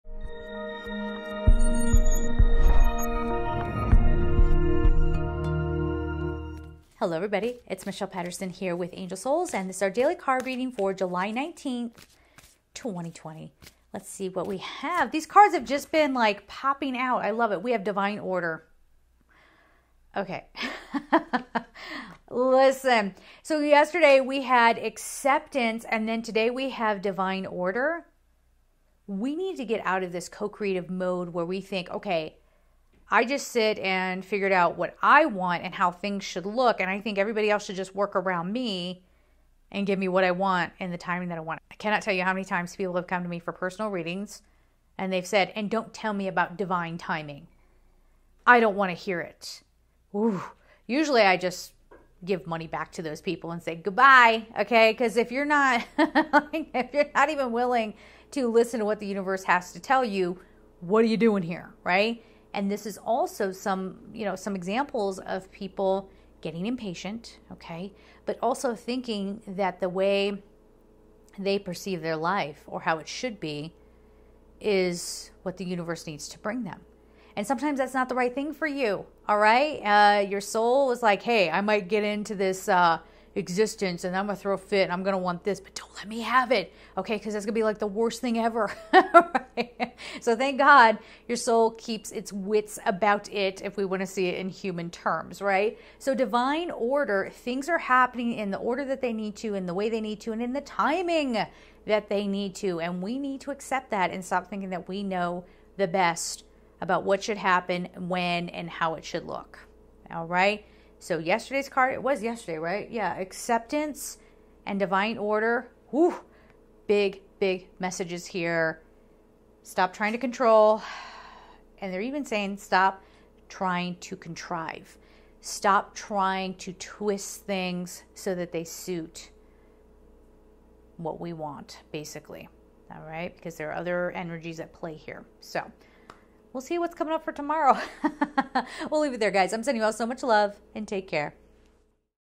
Angel Daily Message July 19, 2020
Decks Used: Angels of Atlantis Oracle Cards: Receive Inspiration and Healing from the Angelic Kingdoms by Stuart Pearce

Ask an Angel Oracle Cards by Toni Carmine Salerno

**Please do not send items without my consent. Thank you.